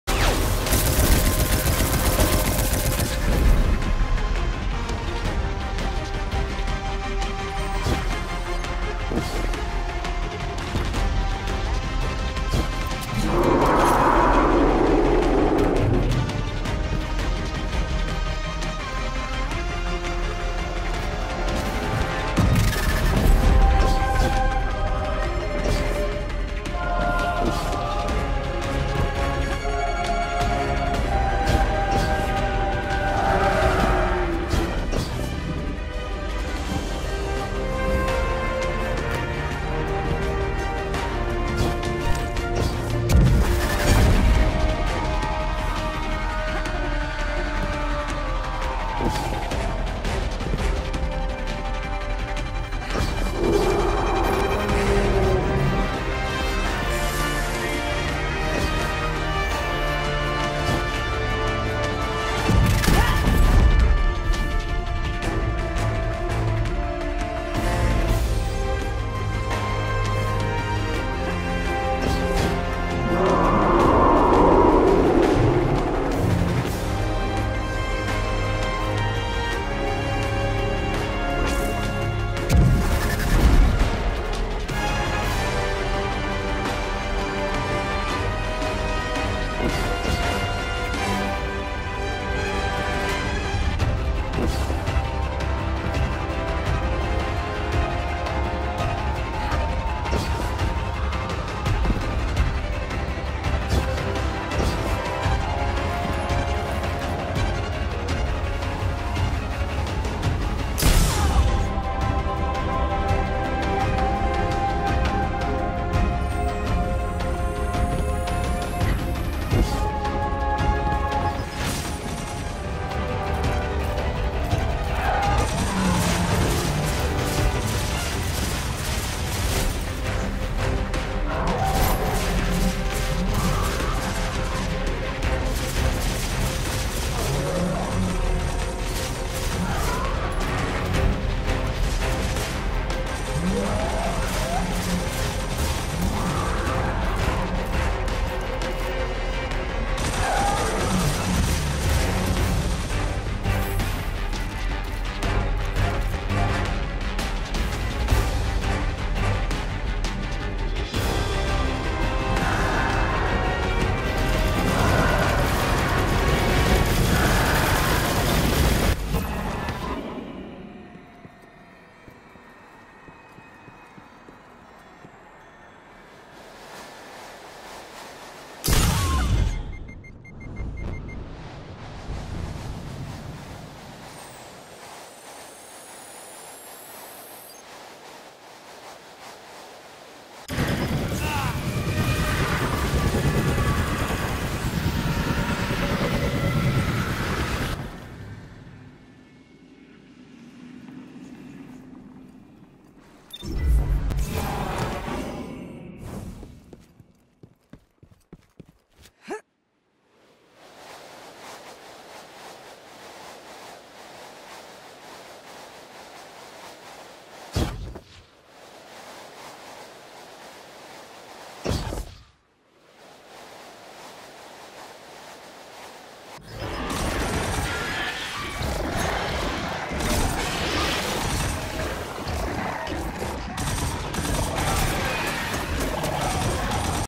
What's a death plane?
Oh, just casually teleporting under the map and being unable to escape, what's up with you?